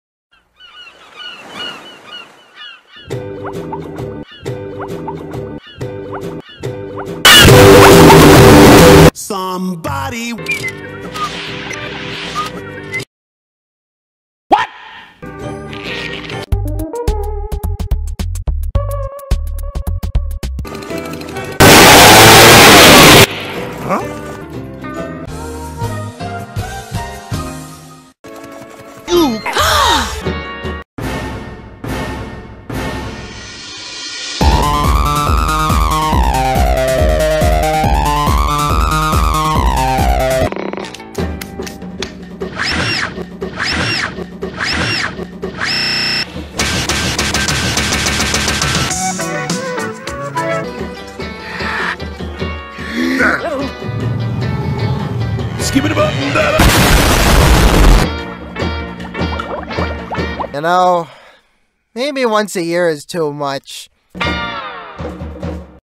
YTP Tennis - YouTube V.S. Silver The Beef Bower (Round 1)
Round 1/6 vs Silver the Hedgehog.